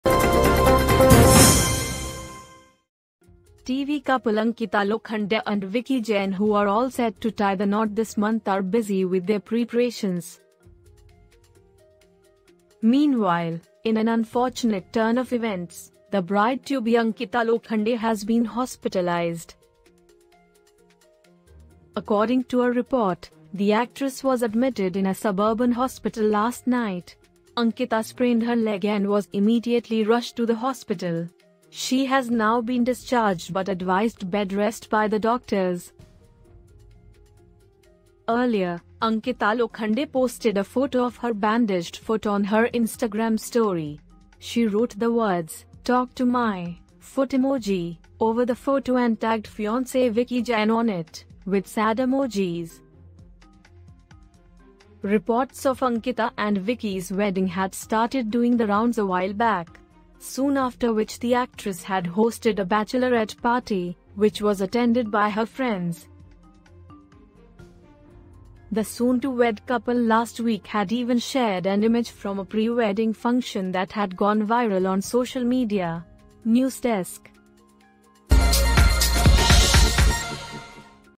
Ankita Lokhande admitted to hospital ahead of her wedding
TV couple Ankita Lokhande and Vicky Jain who are all set to tie the knot this month are busy with their preprations. Meanwhile, in an unfortunate turn of events, the bride-to-be Ankita Lokhande has been hospitalised!